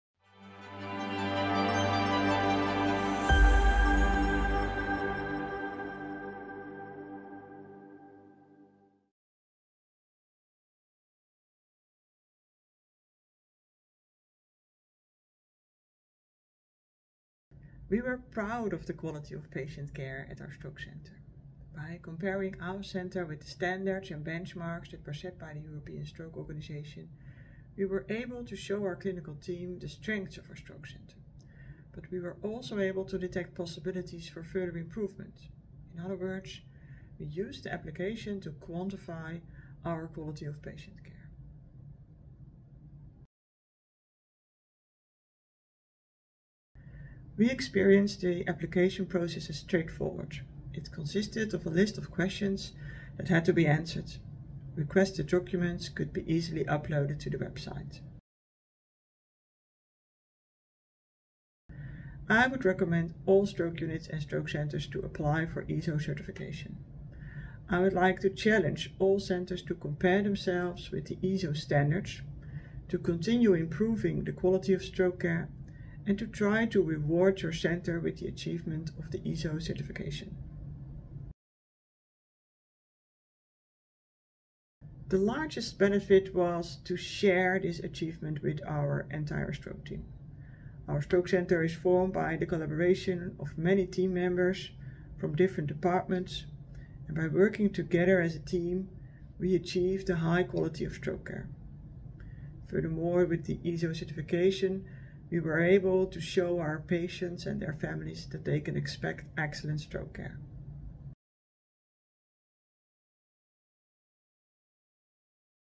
ESO Stroke center certification PM Janssen
PM Janssen speaks about getting the ESO certification for her hospital.